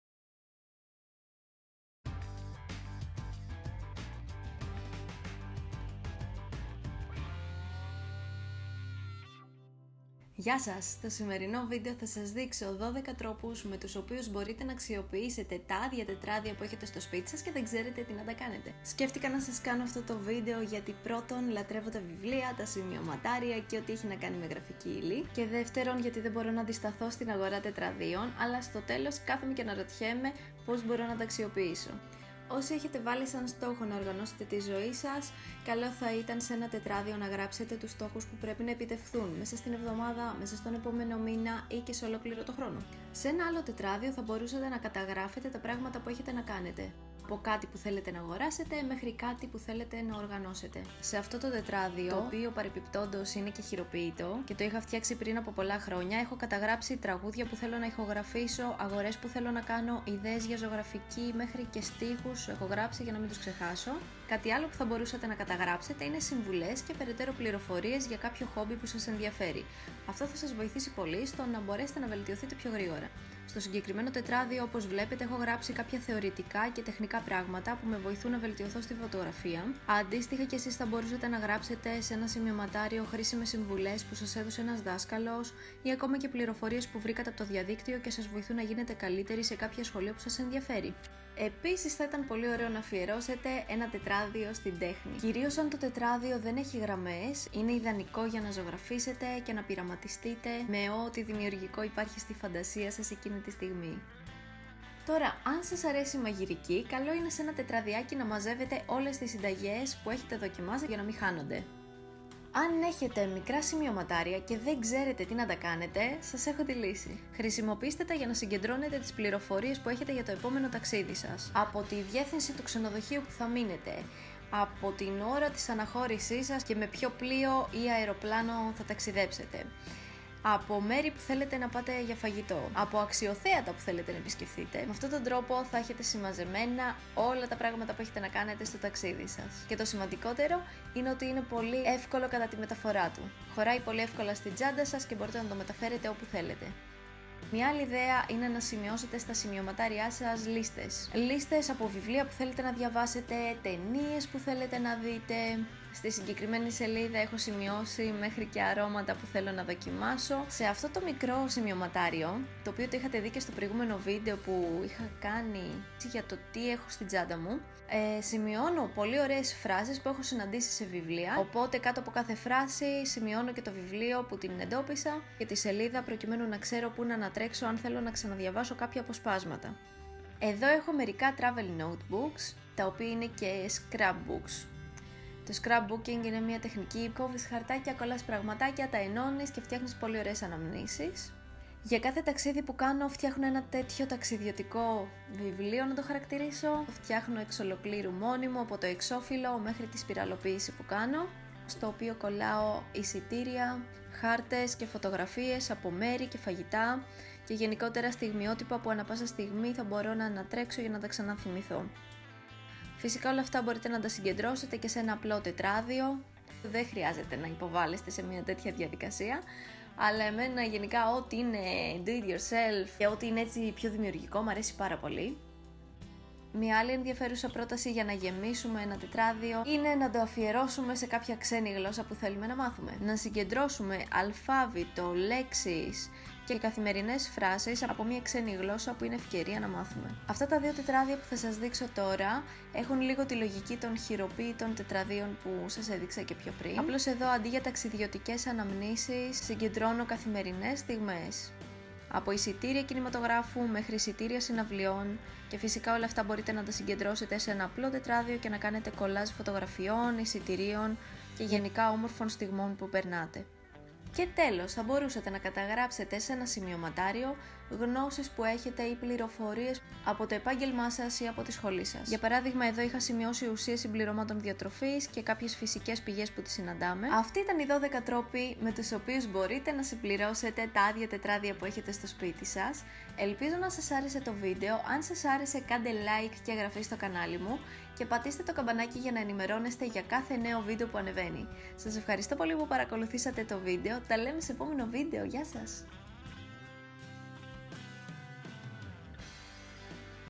Ways To Fill Your Notebooks!
Άν έχετε άδεια τετράδια, αλλά δεν έχετε ιδέες για το πώς μπορείτε να τα αξιοποιήσετε, τότε αυτό το βίντεο είναι για εσάς!

Περιμένω να μου γράψετε στα σχόλια τους τρόπους με τους οποίους χρησιμοποιείτε κι εσείς τα τετράδιά σας!!!